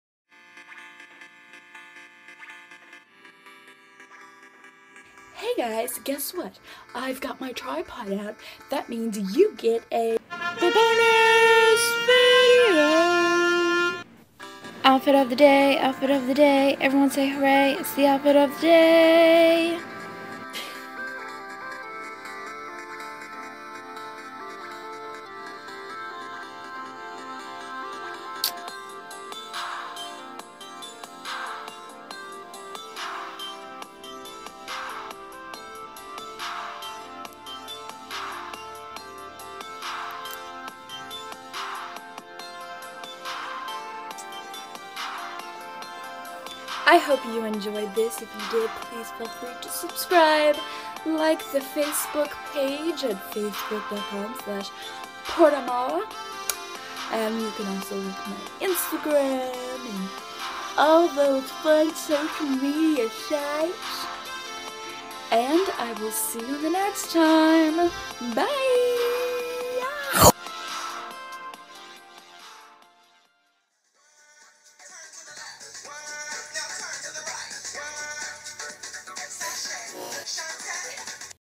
90's Fall Casual (OOTD #3)
Black sweater..target $30
Jeans..goodwill
Black fishnets
Boot..Jeffery Campbell
Pendent.hobby lobby
Music destery smith